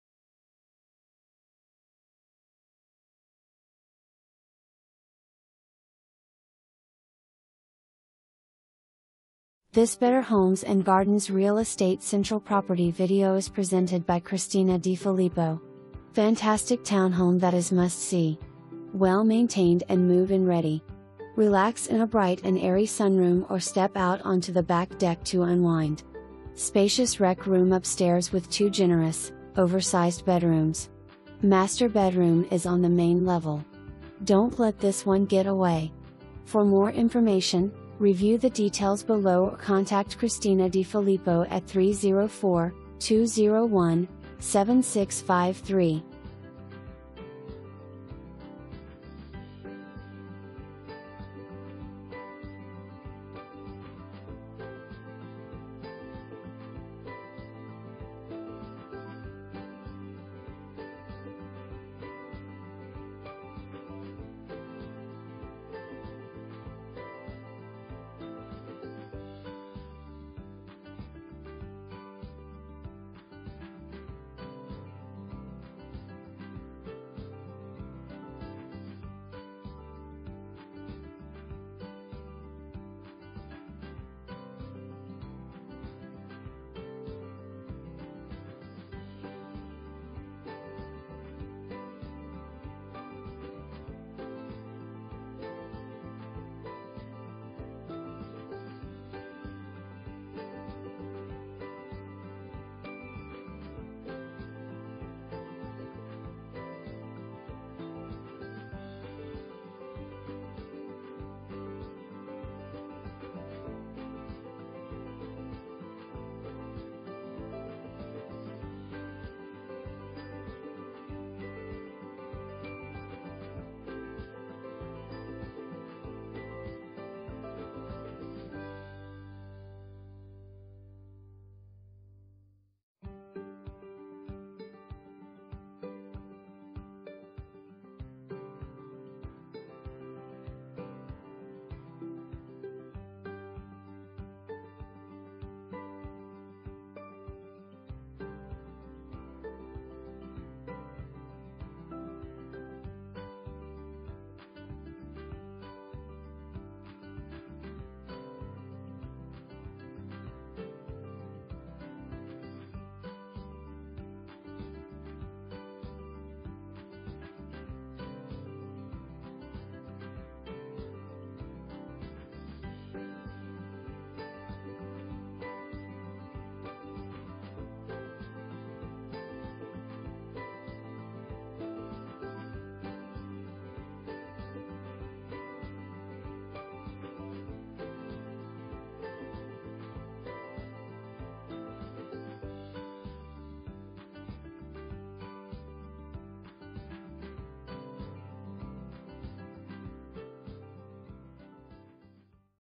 Better Homes and Gardens Real Estate Central - 10 Dapplewood Road
Fantastic townhome that is must see! Well maintained and move-in ready. Relax in a bright and airy sunroom or step out onto the back deck to unwind. Spacious rec room upstairs with two generous, oversized bedrooms. Master bedroom is on the main level. Don't let this one get away!

2 full bath, 1 half bath

Christina DiFilippo
Better Homes and Gardens Real Estate Central